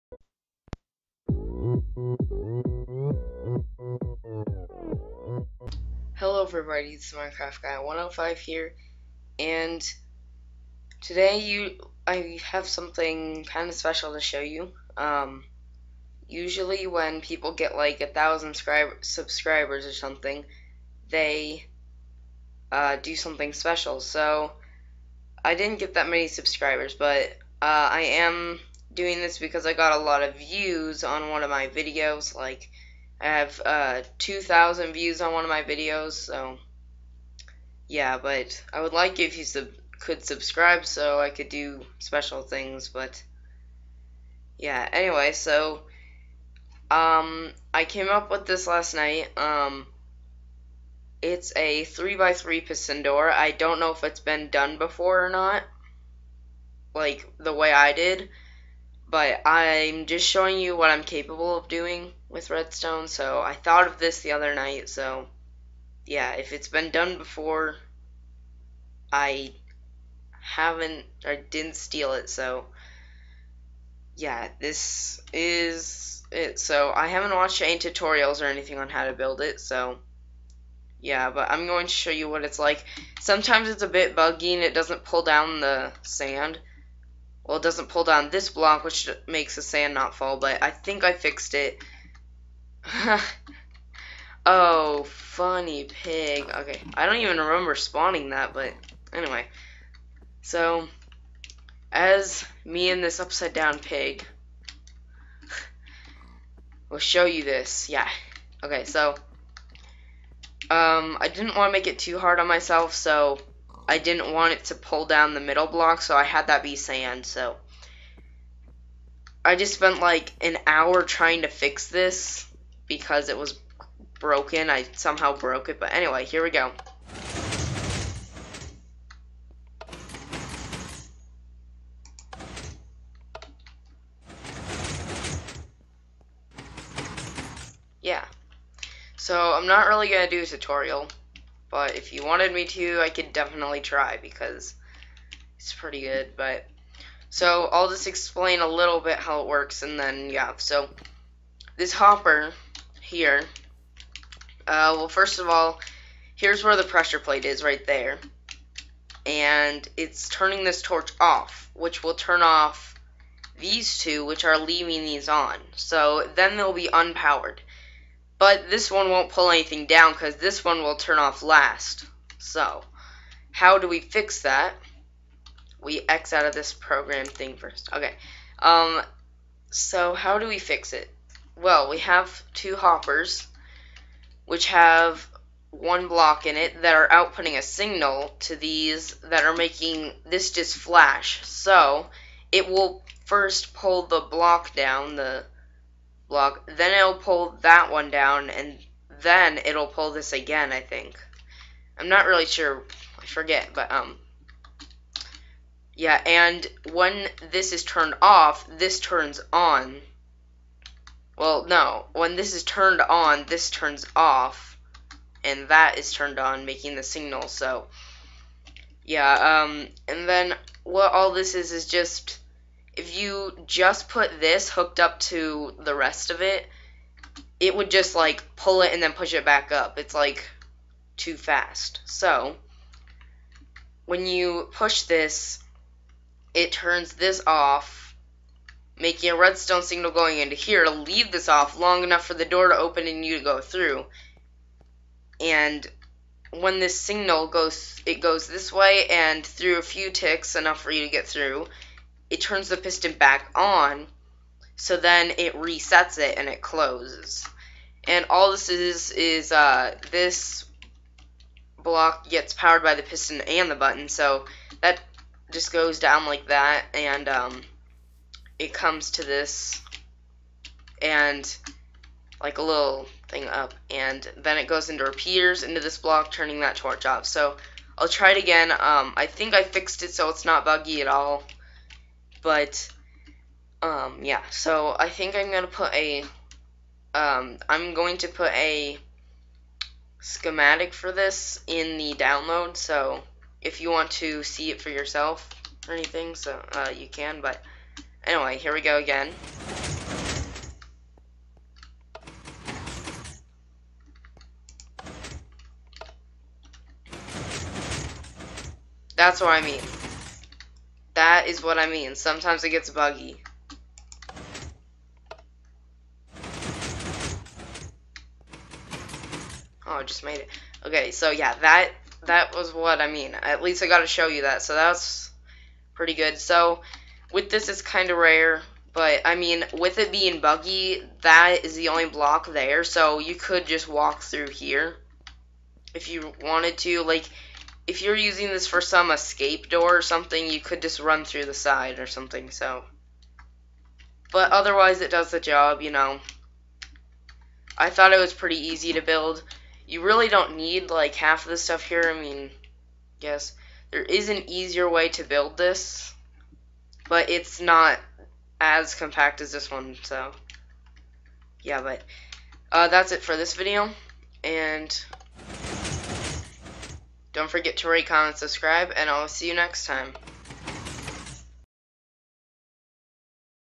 3x3 piston door!
In this video I show my 3x3 piston door. it was pretty easy and I haven't seen it done before but I haven't watched really any videos on 3x3 piston doors so maybe it has but anyway this is mainly to show you what im capable of doing with redstone.